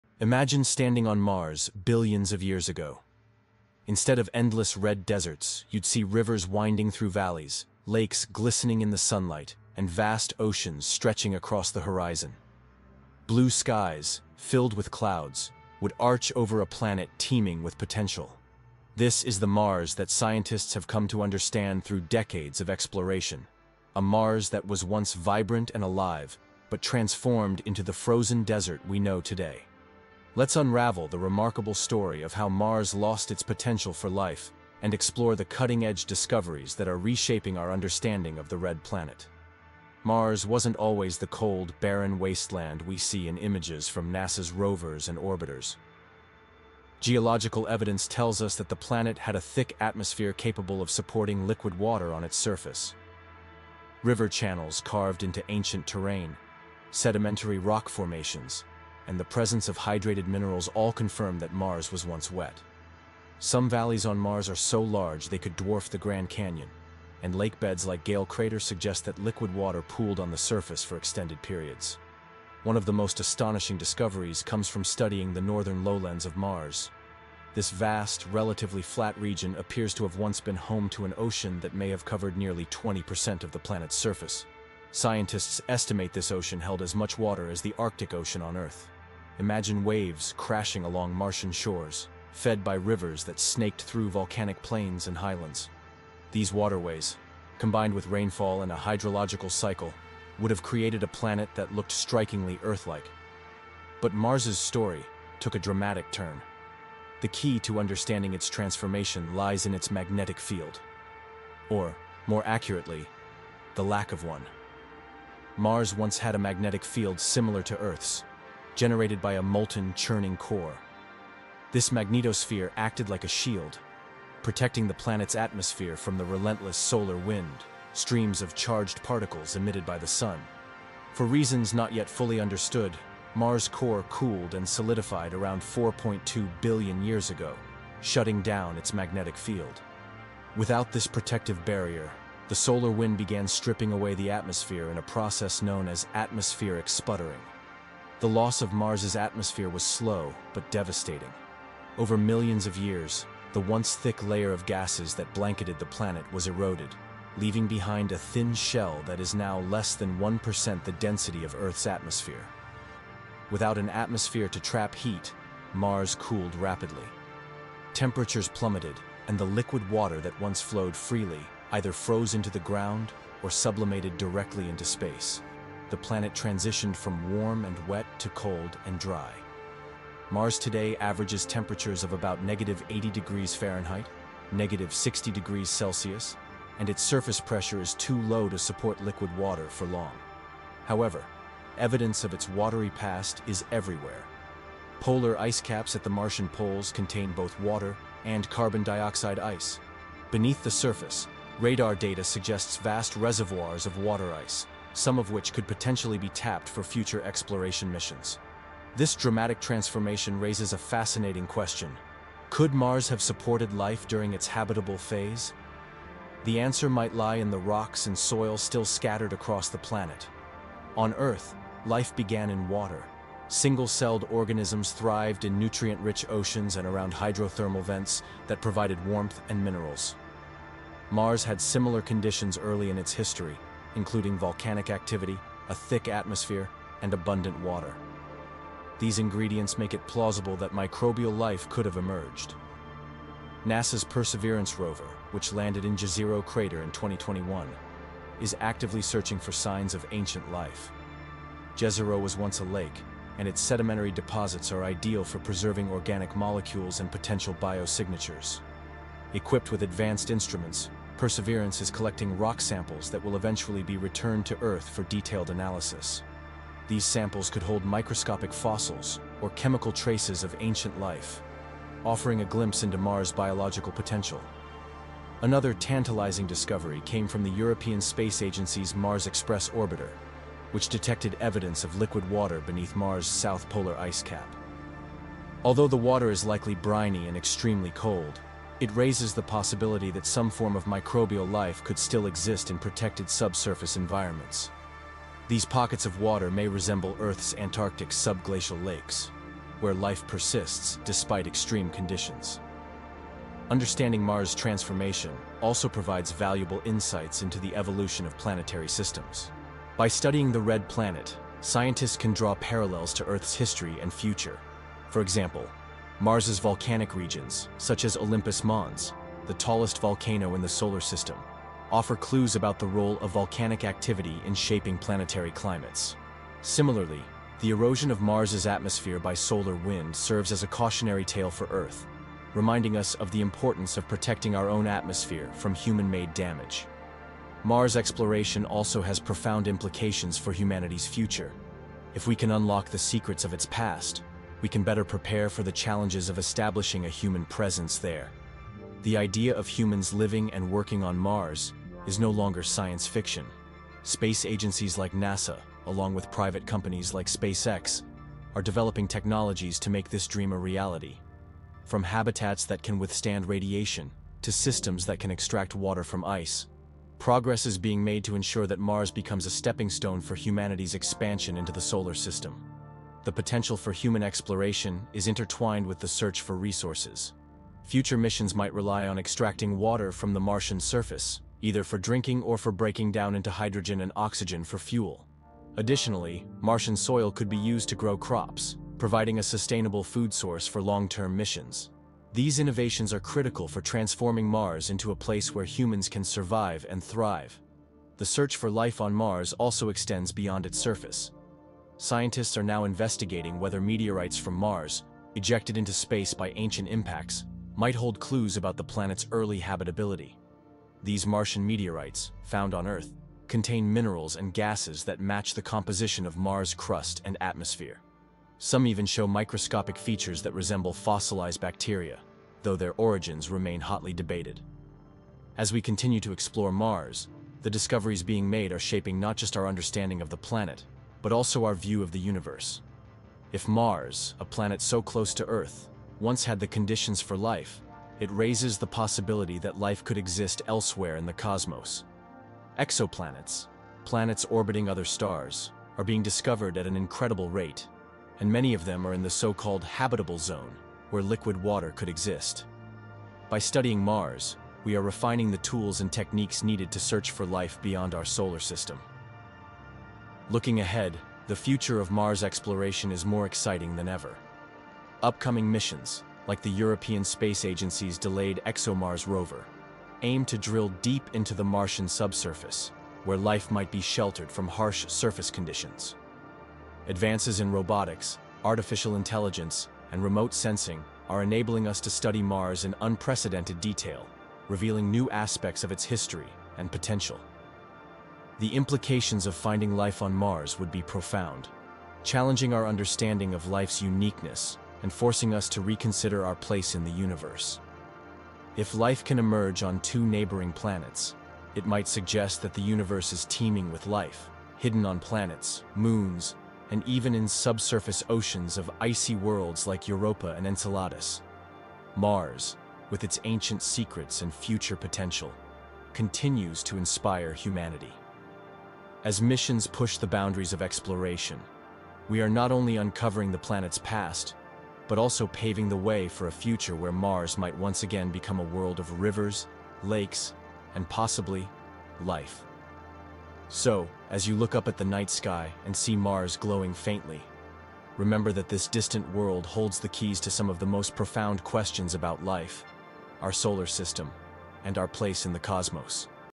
What Did Ancient Mars Actually Look Like? EARTH 2.0?
Imagine standing on Mars billions of years ago. Instead of vast dry desert, you'd see rivers and valleys, vast oceans glimmering on the horizons and large blue clouds arching over a planet teeming with potential. This is the once vibrant Mars that was alive, but how did it lose it potential for life? and what can this teach us about the future of our planet?

Love all things space? Check out our space themed store at cosmicospace.com